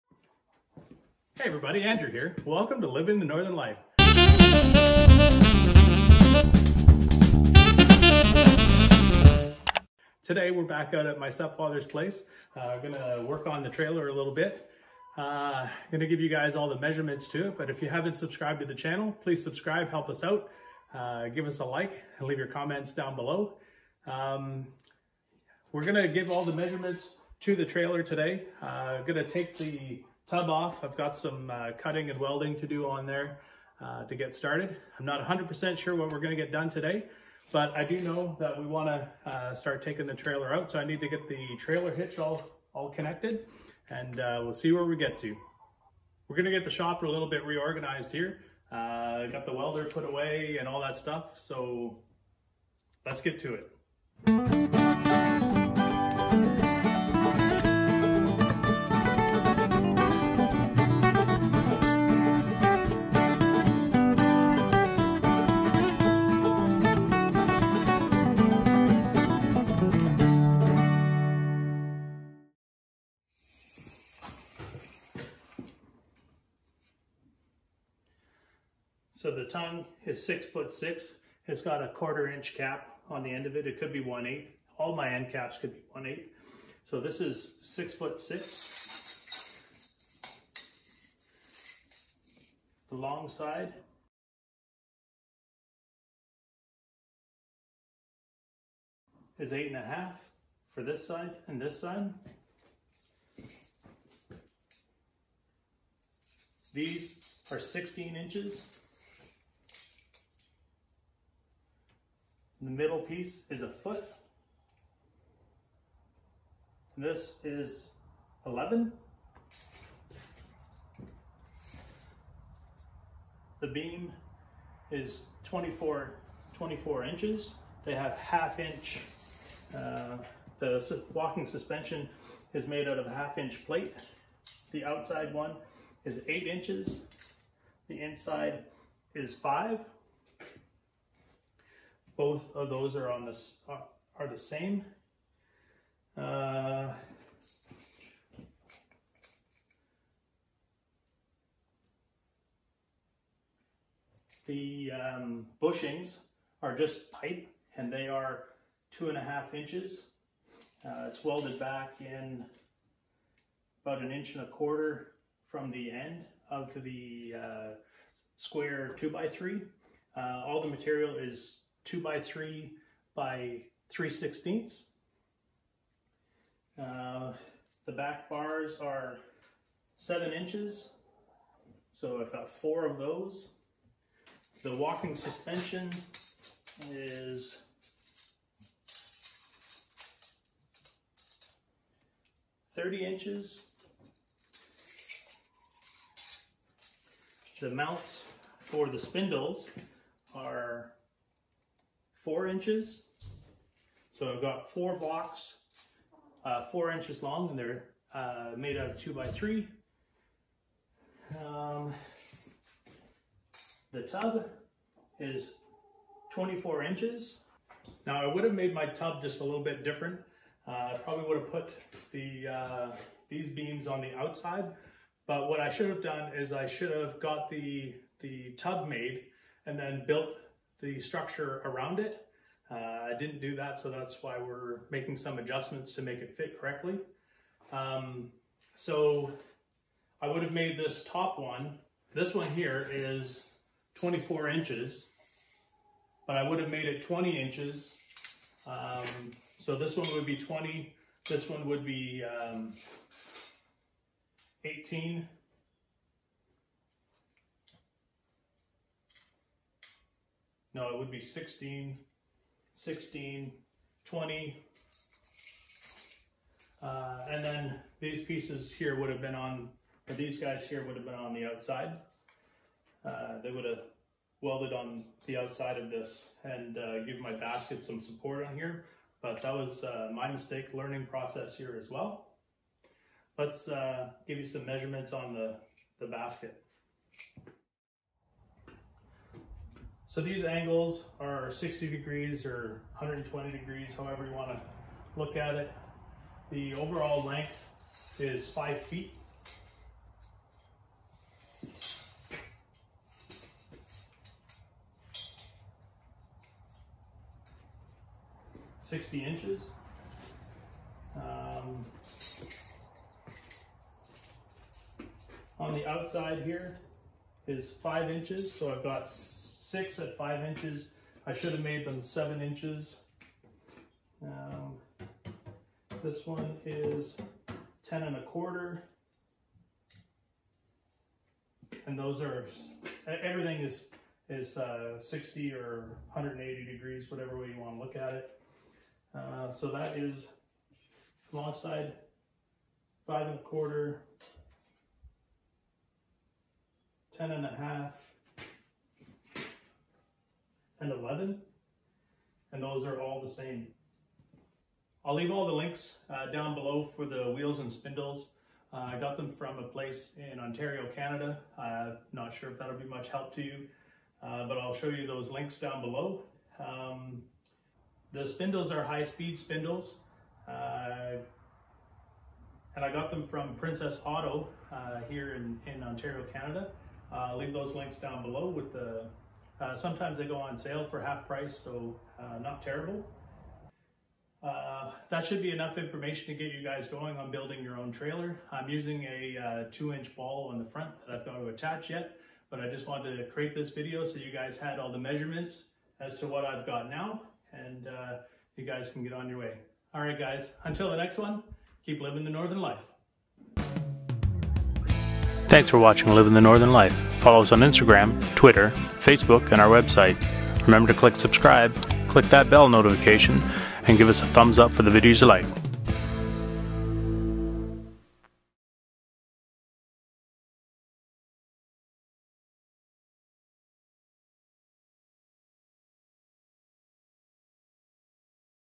The Ultimate Quad Trailer - Dimensions - Part 9
Dimensions for the Ultimate Quad Trailer and the Walking Suspension.

I bought these in Ontario Canada so I'm not sure these will be much help.

Base is 2"x3"x3/16"
Bed is 2"x2"x1/8"

9 inch Chainsaw Tooth Brush Blade

Books
Learn to Timber Frame

Tools
Swanson Tool SO107 12-Inch Speed Square

0:00 Intro
0:10 Plans
6:00 Closing Remarks
06:48 Outro